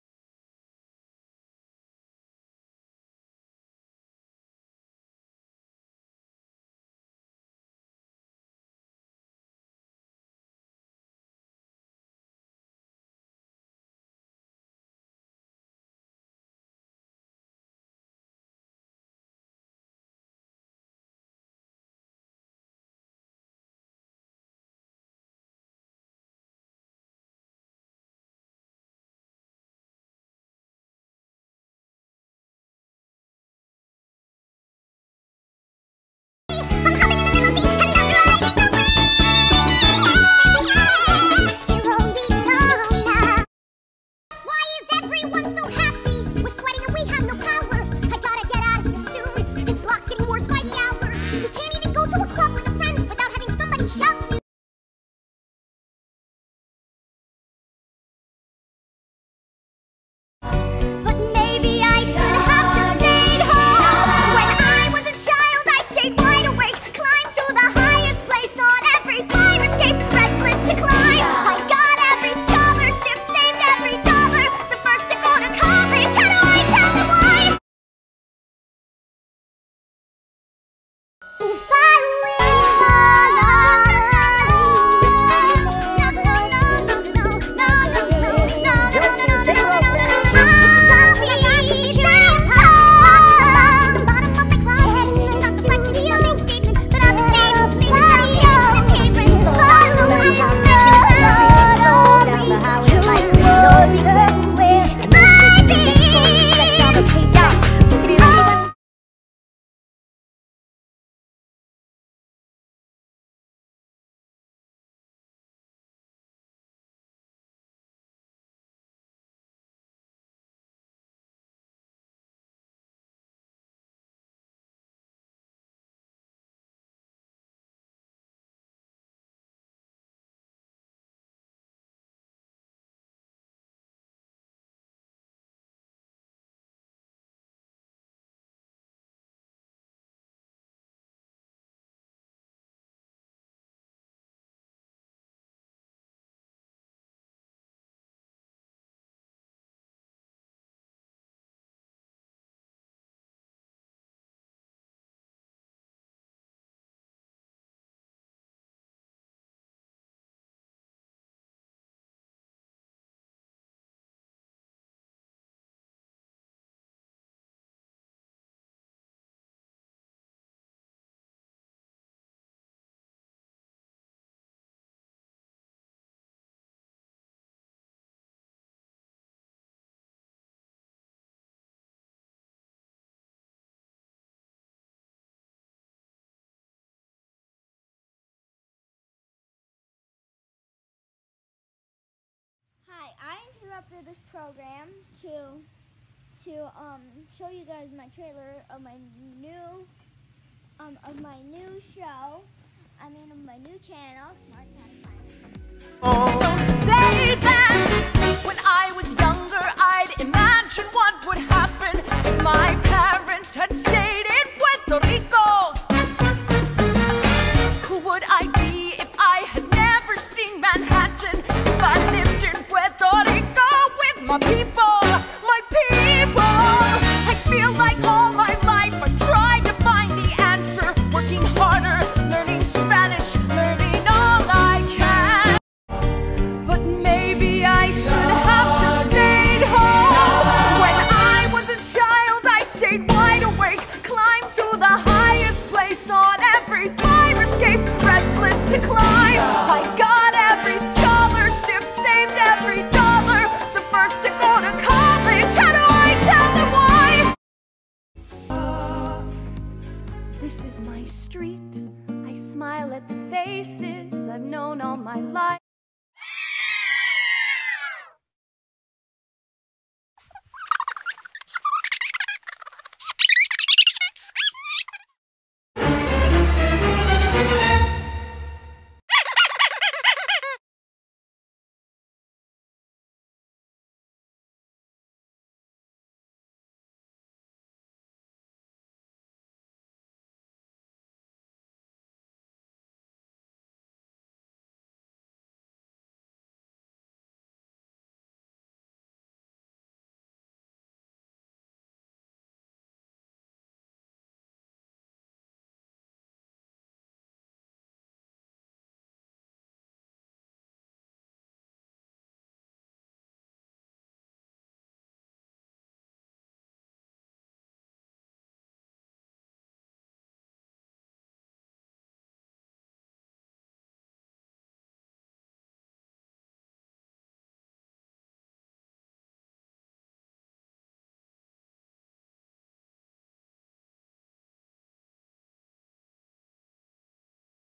Magical Besties Season 1 episode 2
Gab Gabs is smart sooo smart she knows that it is going to be a perfect day. And she was right!